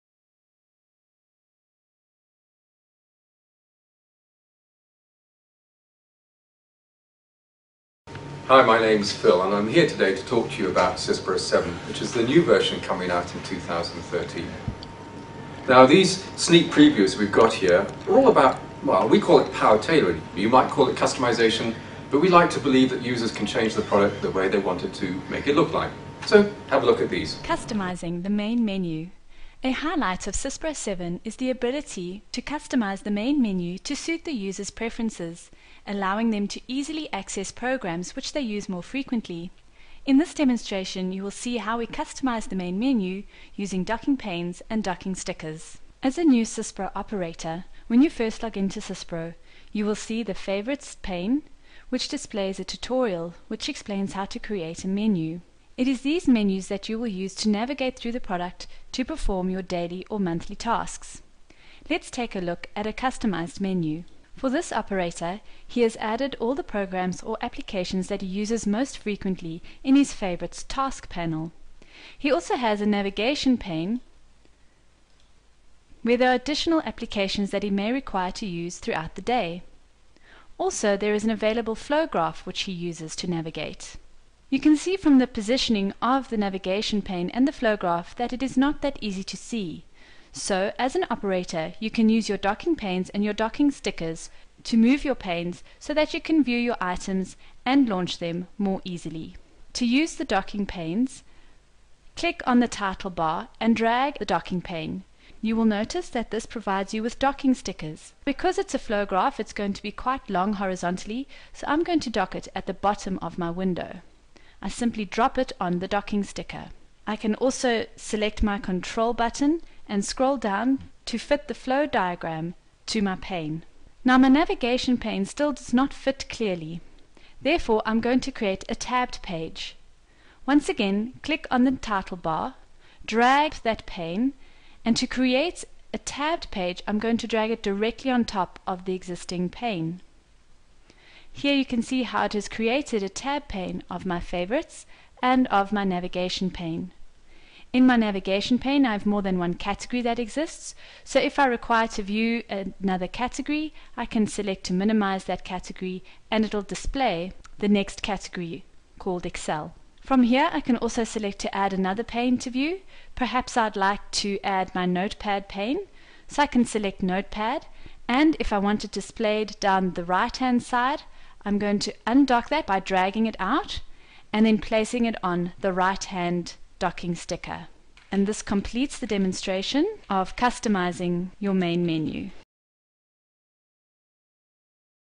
SYSPRO ERP Software Sneak Preview - Customizing the Main Menu in SYSPRO 7
** For optimal viewing select 720p HD quality **
Styled along the lines of Windows 8, SYSPRO 7 brings active tiles and touch capabilities to its enterprise UI, enabling you to easily create tailor-fit environments.